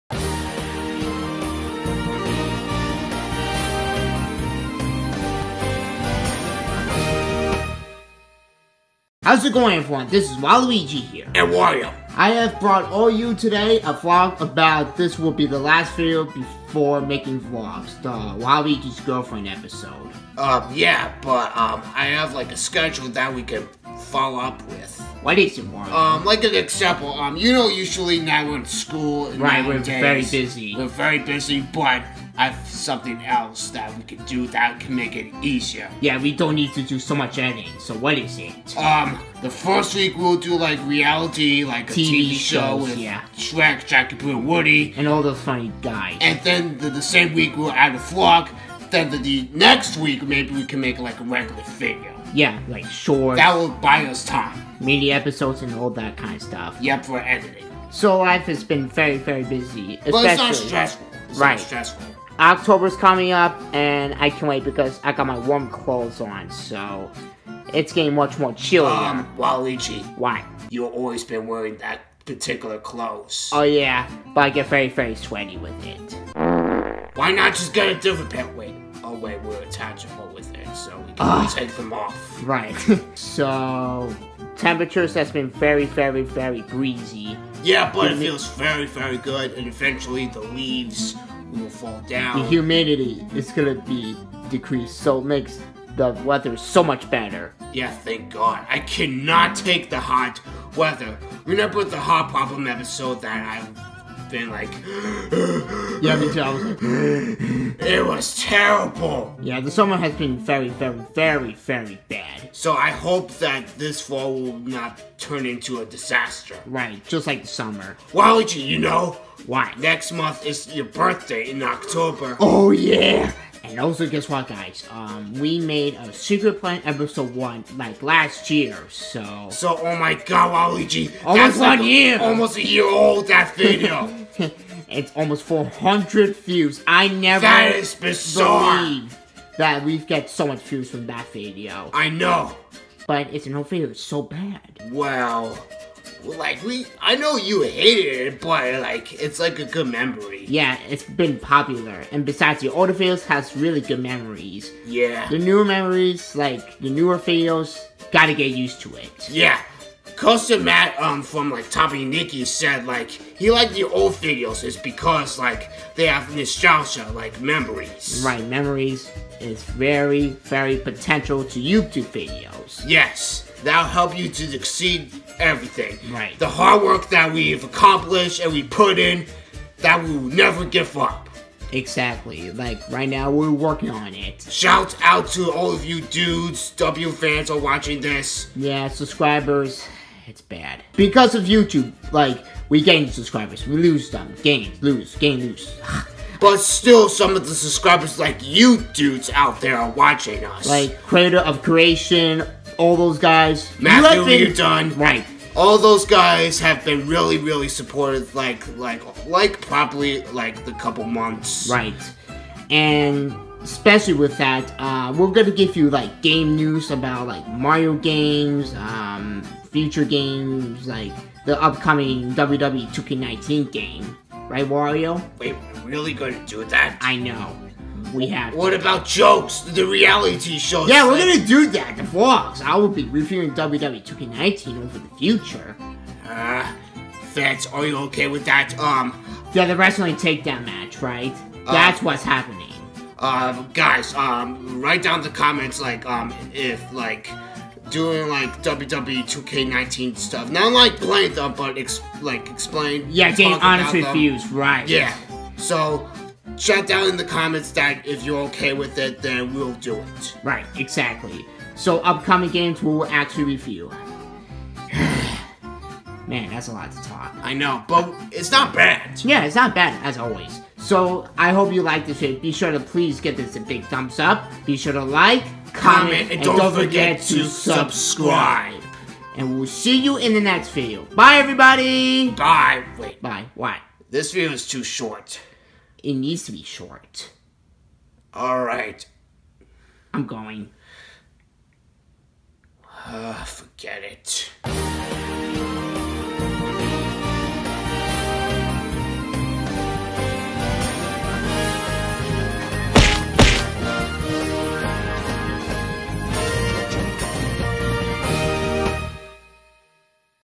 Wario and Waluigi Vlog: Coming!
The Waluigi's Girlfriend Episode is coming very soon about next week so hang tight everyone! In this Vlog we're talking about upcoming news and updates in our channel which we can review and react to new upcoming games like bunch of Mario games, WWE 2K19 and etc.